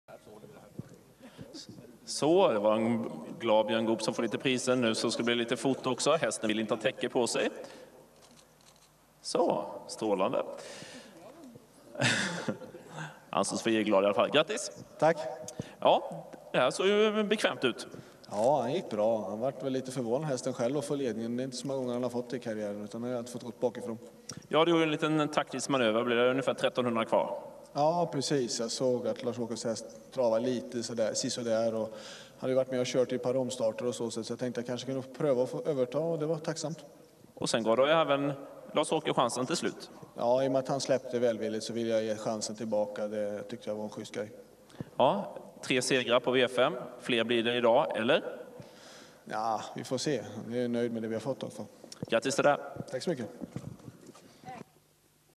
Segerintervju lopp 6, 120608
Segerintervju med Goop Bj�rn efter segern med ARABIAN MYSTERY, i lopp 6 pa farjestadstravet den 120608.